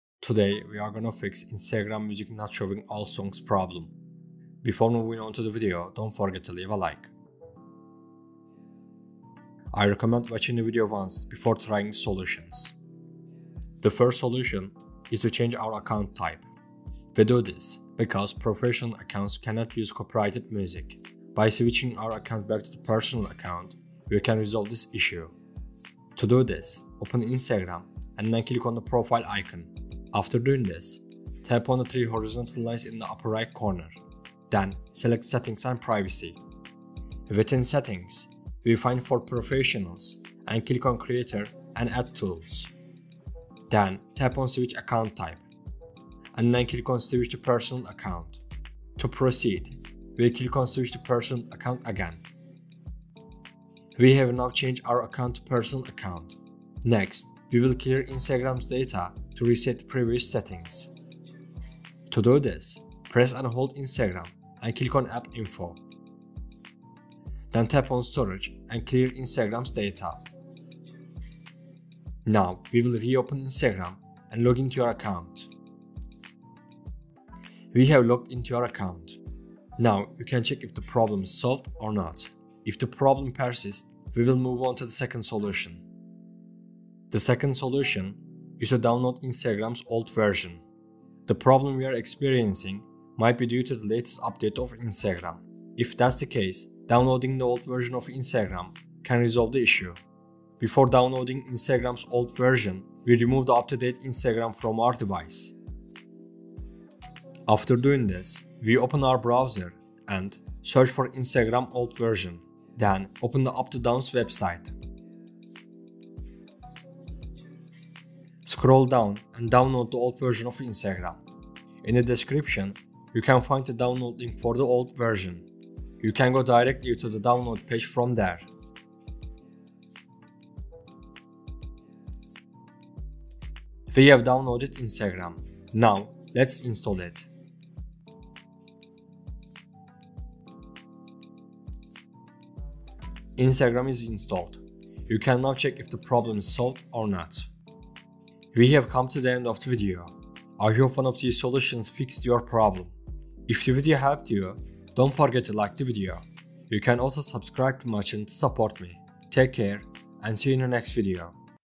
FIX INSTAGRAM MUSIC NOT WORKING | Instagram Music Not Showing All Songs Fix
Here's instagram music not working 2023. This error is searched as instagram music not showing all songs and this song is currently unavailable instagram story. And in this video I showed you how to fix instagram story music not working problem.

Video Parts:
00:00 Info&Intro
00:13 Change account type
01:26 Download Instagram’s old version
02:28 Ending&Outro